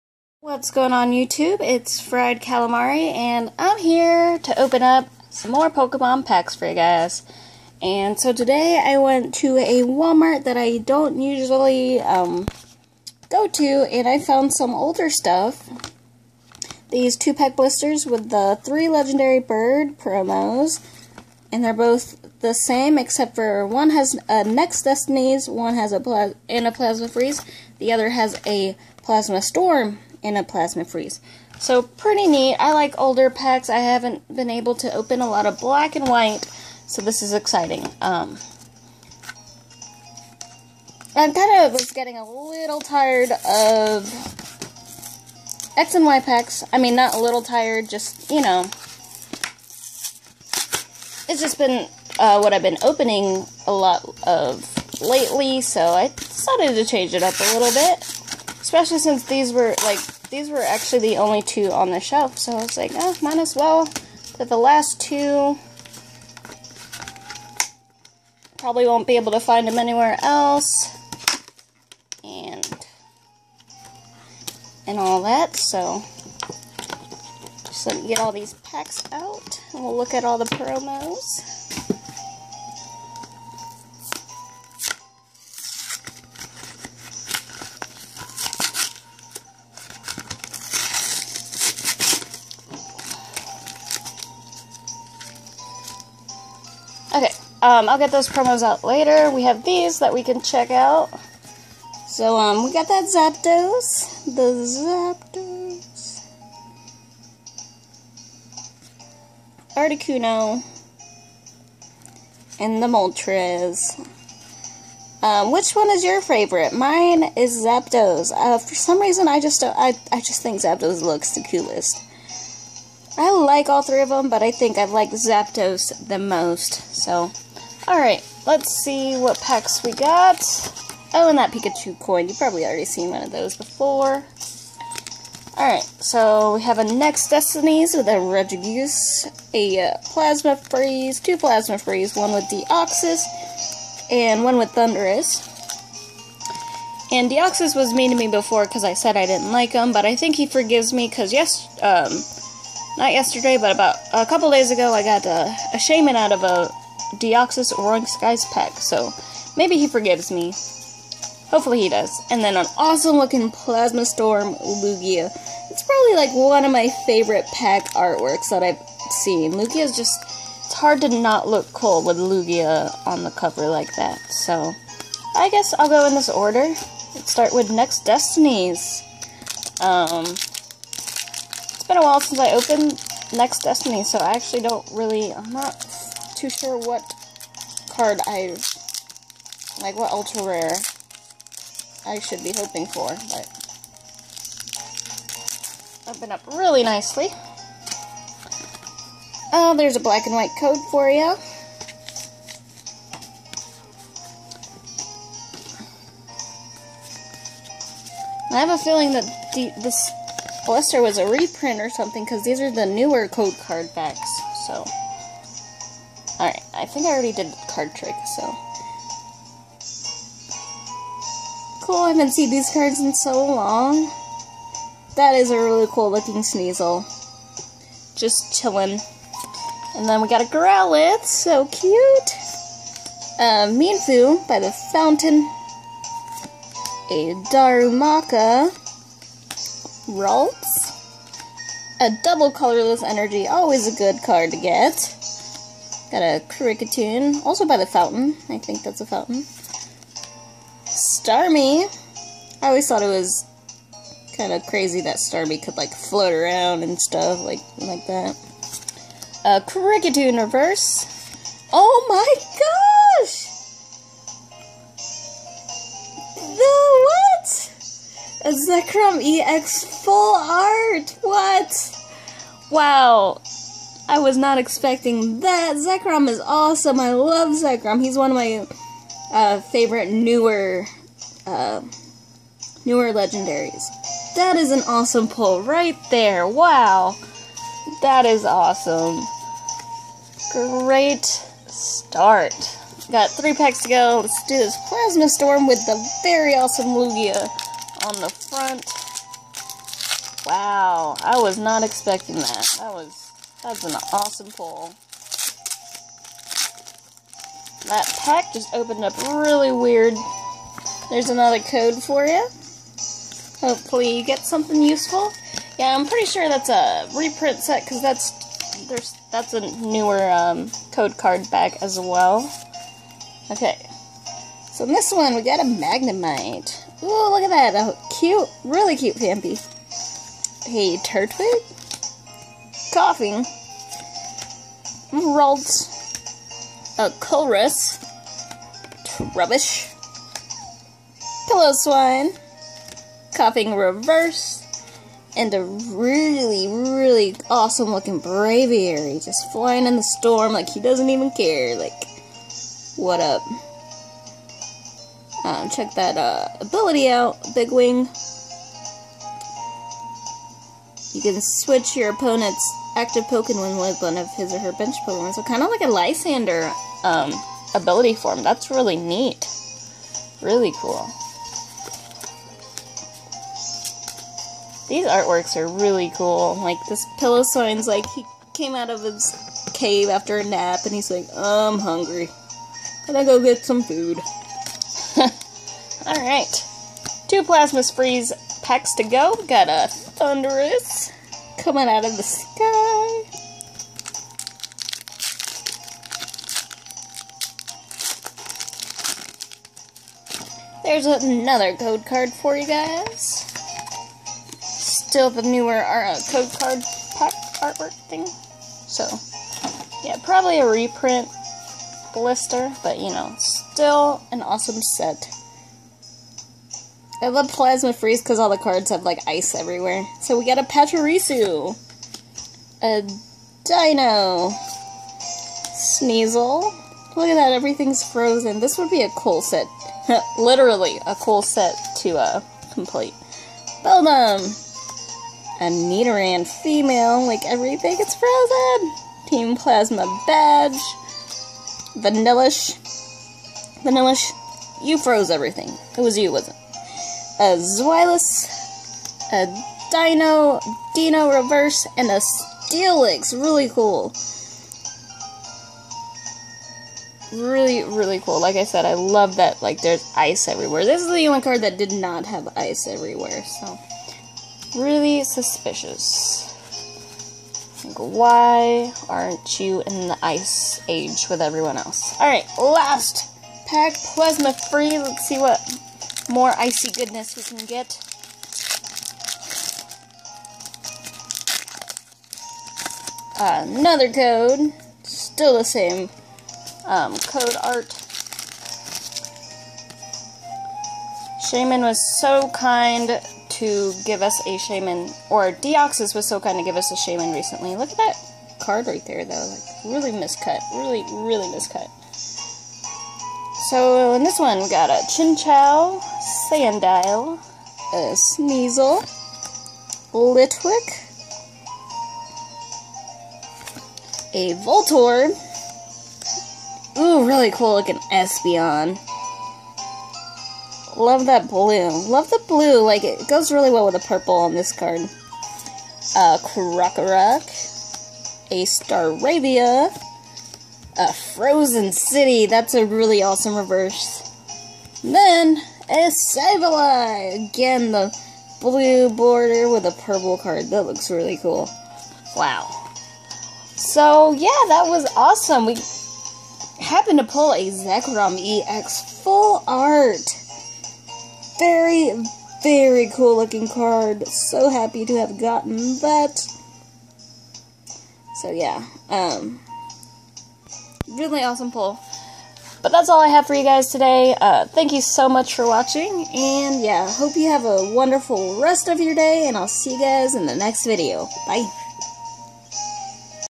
Pokemon tcg-Black and White series Opening!!!
Opening up a couple blisters of Black and White series cards! In these blisters were a set of the 3 legendary birds alternate holo cards. And 2 Plasma Freeze, 1 Plasma Storm, 1 Next Destinies! Let's see what pulls we can get today! :D

Thanks for checking out the description! Here's a free code!:QY7STQ4E5VWKR